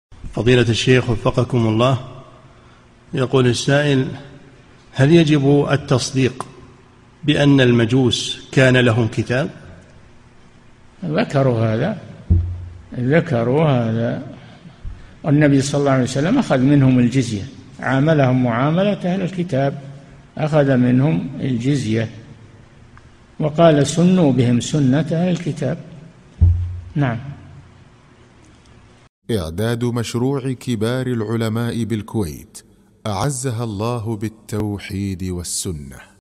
[2853 -3022] هل كان للمجوس كتاب؟ - الشيخ صالح الفوزان
-"[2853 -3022] السؤال: هل يجب التصديق بأن المجوس كان لهم كتاب؟
الجَوَابُ: ذكروا هذا، ذكروا هذا، والنبي ﷺ أخذ منهم الجزية، عاملهم معاملة أهل الكتاب، أخذ منهم الجزية، وقال سنوا بهم سنة أهل الكتاب.
للاشتراك في قناة الشيخ صالح الفوزان

للاستفسار والإقتراحات والدعم
رئيس الفريق
لمشروع كبار العلماء